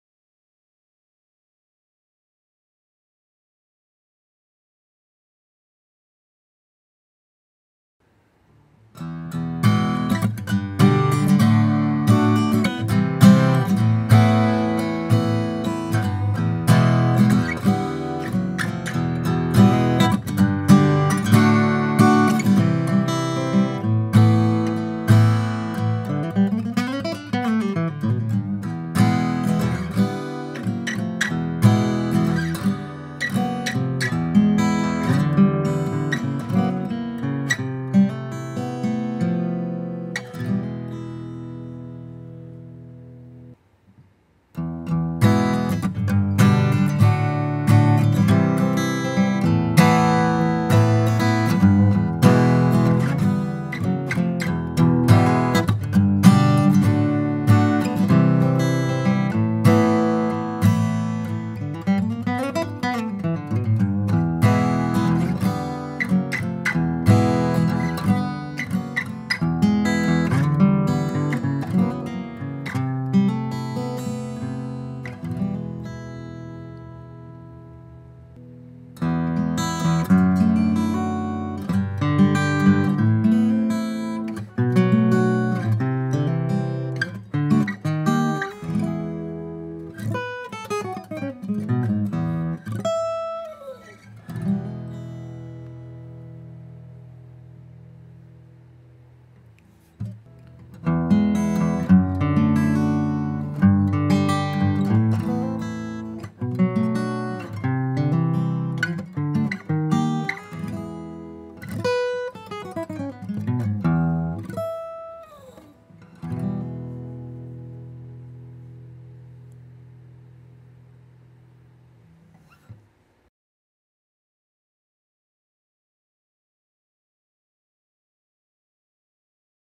Comparison by Request: Martin Custom Shop OM 41/45 and Huss and Dalton TOM-R Custom
I receive requests periodically to compare guitars.  This is a comparison between two very high end OM guitars:  a Huss and Dalton TOM-R custom, with thermo-cured red spruce and cocobolo, and a Martin Custom Shop OM 41/45 with high altitude Swiss Spruce and Guatemalan Rosewood.
0:00 Intro
0:08 Huss and Dalton TOM-R Custom
0:43 Martin Custom Shop OM 41/45
1:18 Huss and Dalton TOM-R Custom
1:40 Martin Custom Shop OM 41/45